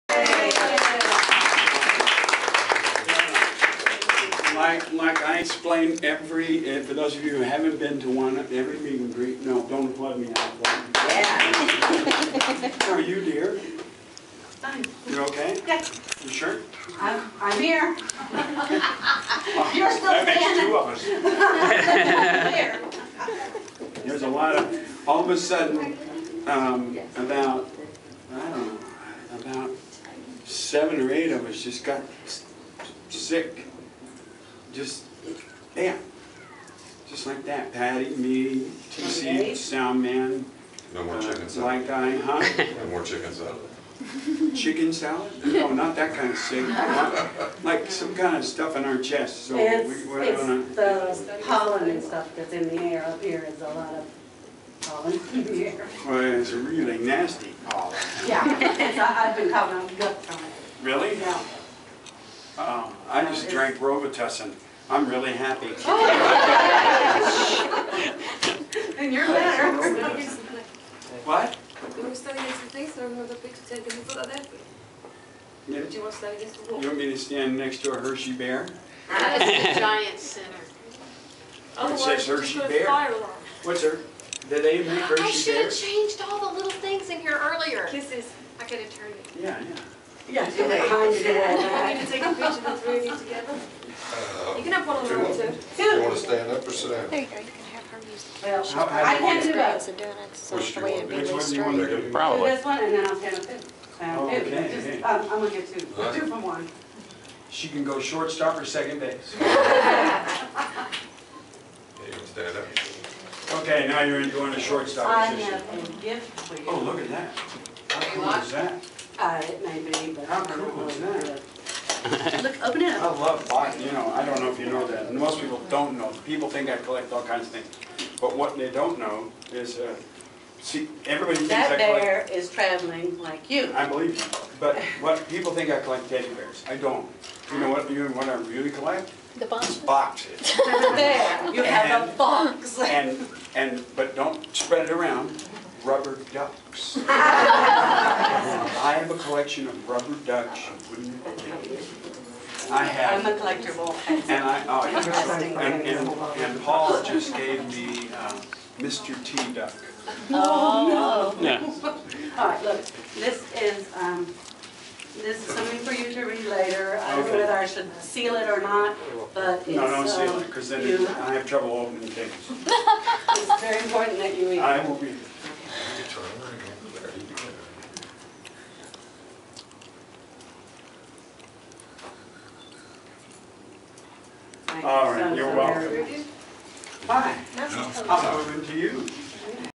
meatloaf meet and greet Hershey PA
Meatloaf wasn't feeling well at the Hershey, PA show, but he still rocked the house! Can't believe he's 62! Geez makes me feel old! And - he collects rubber ducks!